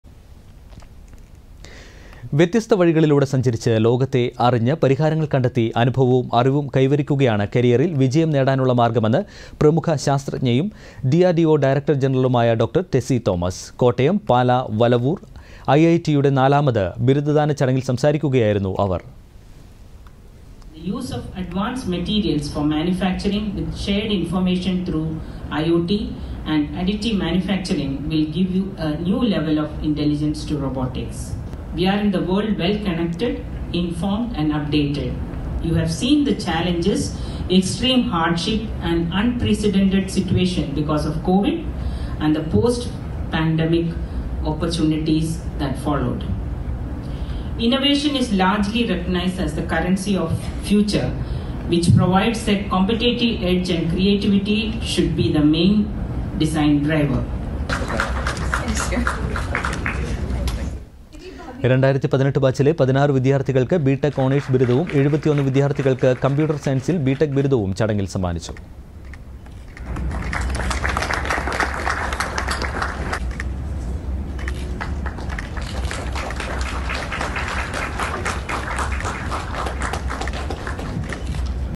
Pala IIIT Convocation ceremony | Tessy Thomas
വ്യത്യസ്ത വഴികളിലൂടെ സഞ്ചരിച്ച് ലോകത്തെ അറിഞ്ഞ് പരിഹാരങ്ങൾ കണ്ടെത്തി അനുഭവവും അറിവും കൈവരിക്കുകയാണ് കരിയറിൽ വിജയം നേടാനുള്ള മാർഗമെന്ന് പ്രമുഖ ശാസ്ത്രജ്ഞയും ഡി.അർ.ഡി.ഓ ഡയറക്ടർ ജനറലുമായ ഡോ. ടെസി തോമസ്.

കോട്ടയം പാലാ വലവൂർ ഐ ഐ ഐ ടിയുടെ നാലാമത് ബിരുദദാന ചടങ്ങിൽ സംസാരിക്കുകയായിരുന്നു അവർ.

2018 ബാച്ചിലെ 16 വിദ്യാർ ഥികൾക്ക് ബിടെക് ഓണേഴ്സ് ബിരുദവും 71 വിദ്യാർഥികൾക്ക് കമ്പ്യൂട്ടർ സയൻസിൽ ബിടെക് ബിരുദവും ചടങ്ങിൽ സമ്മാനിച്ചു.